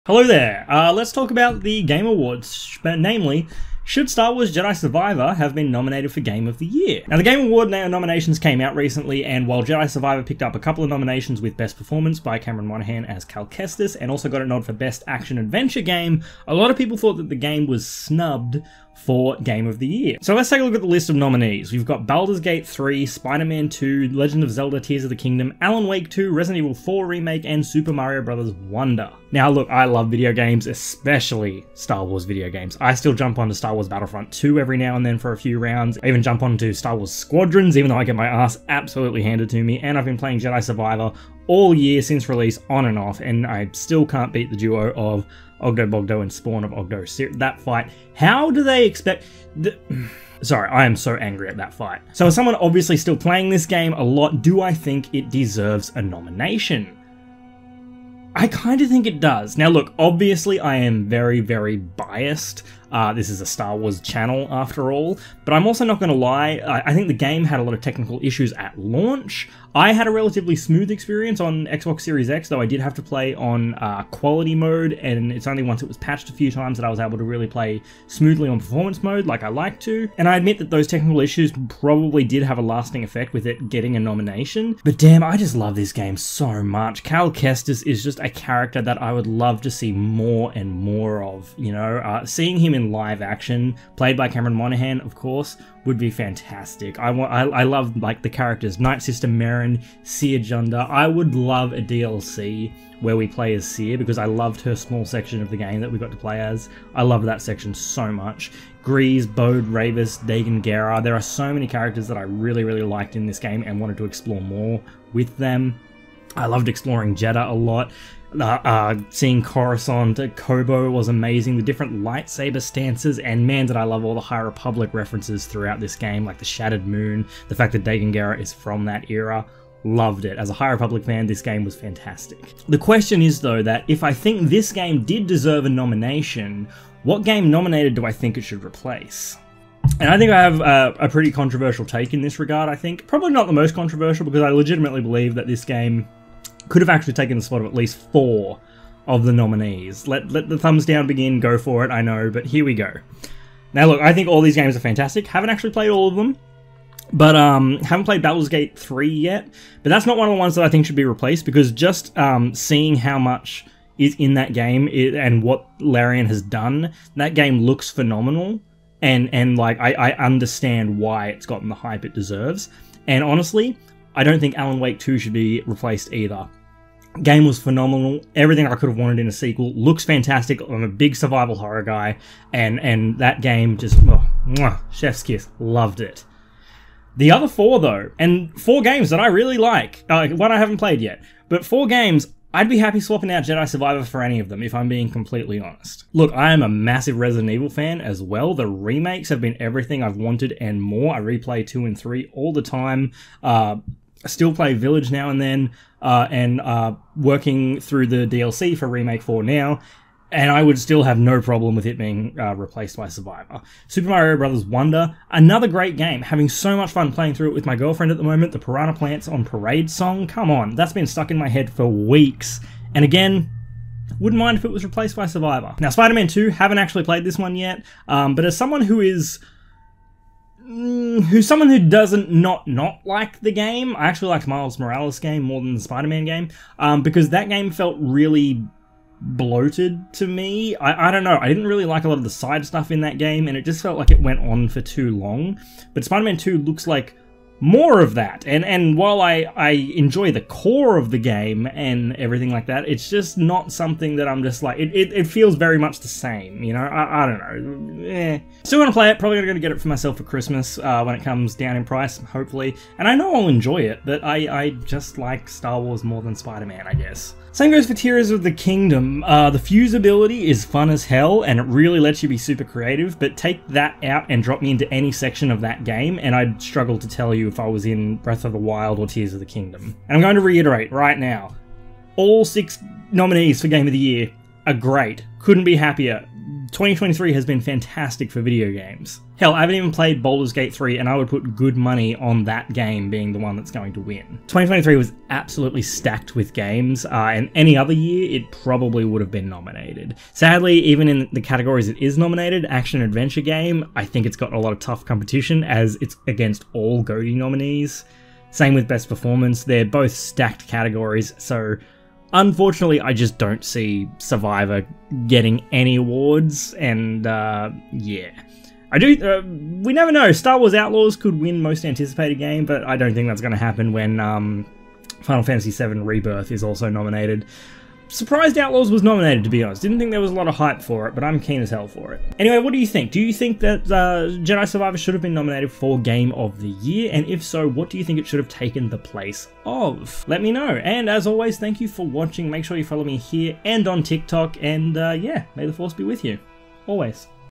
Star Wars Jedi Survivor Should Have Been Nominated for GOTY
Star Wars Jedi Survivor picked up two nominations at the 2023 Game Awards, one for best performance for Cameron Monaghan as Cal Kestis, and one for Best Action/Adventure Game.

But should Jedi Survivor have been nominated for Game of the Year?